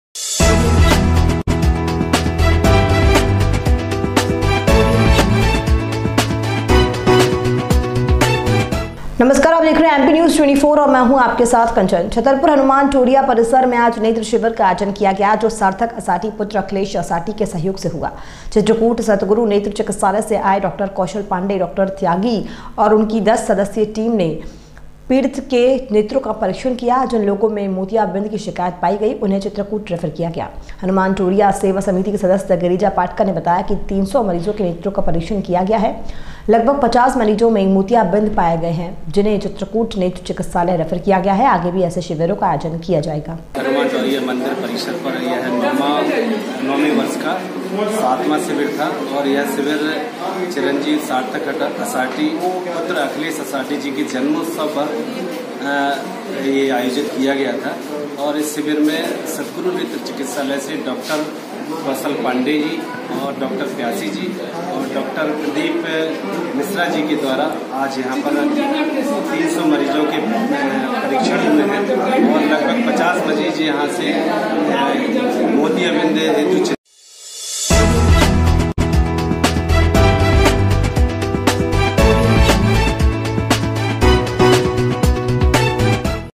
गिरजा पाटकर- एकदिवसीय नेत्र परीक्षण शिविर संपन्न 65 मरीजों के हुए ऑपरेशन
26 जुलाई 2019  छतरपुर